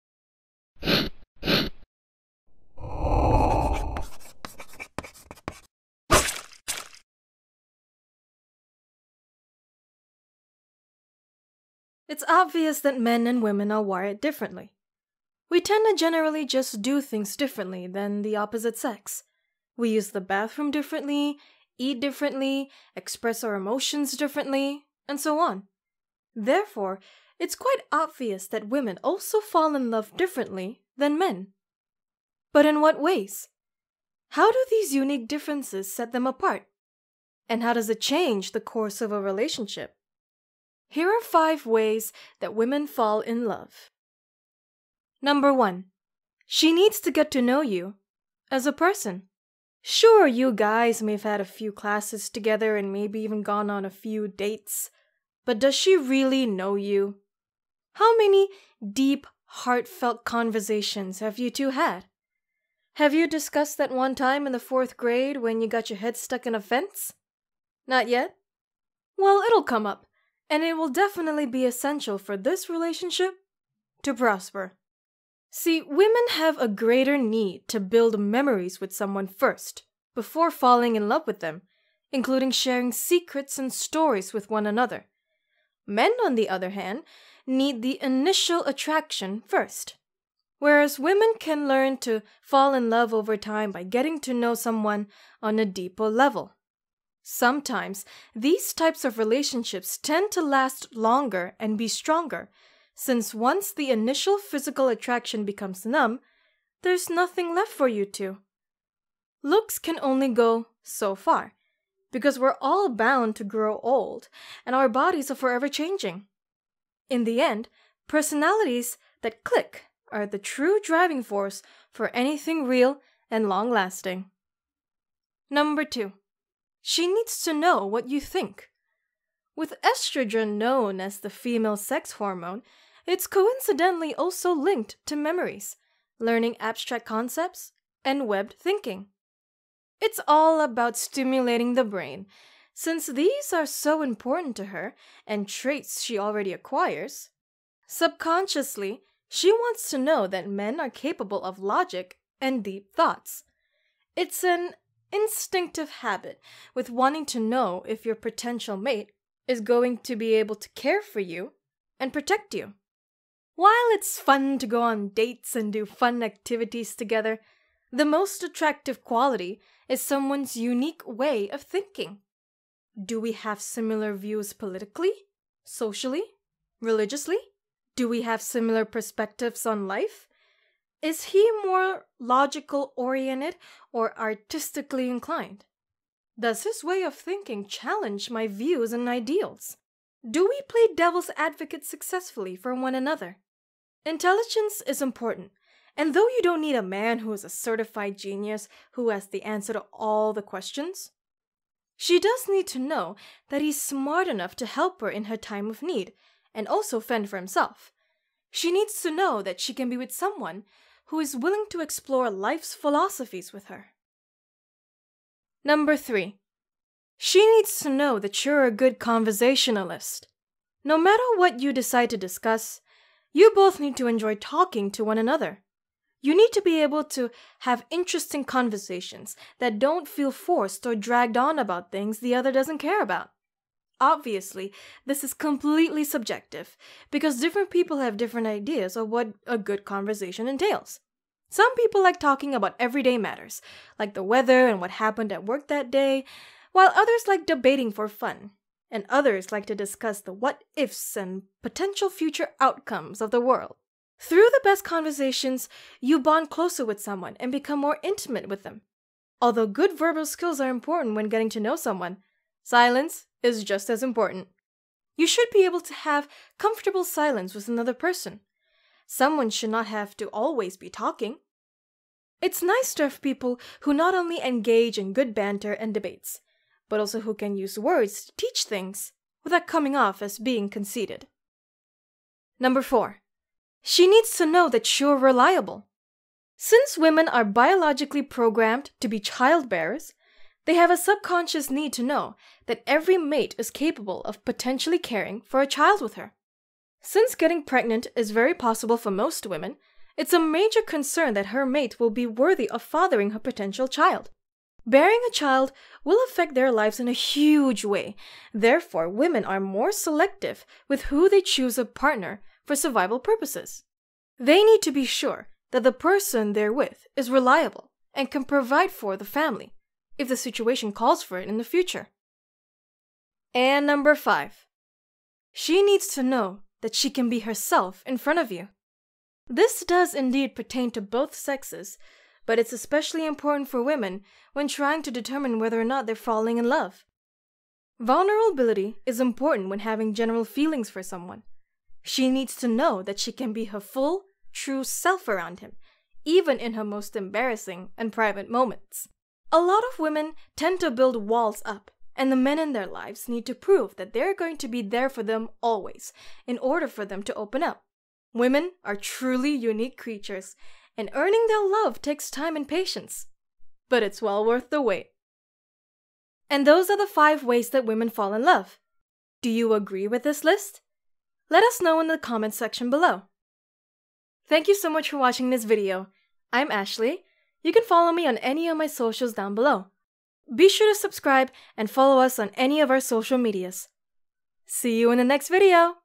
5 Ways Women Fall in Love
Men and women are wired differently. And this rule is no exception in the department of love. Therefore, women also fall in love differently than men. But, what are these unique differences that set them apart?

Script: Melissa Rorech
Voice Over: Ashleigh Aishwarya
Animation: Joleen Oltmanns

Feedback Editor: Risha Maes
Project Manager: Erin Bogo
Producer: Psych2Go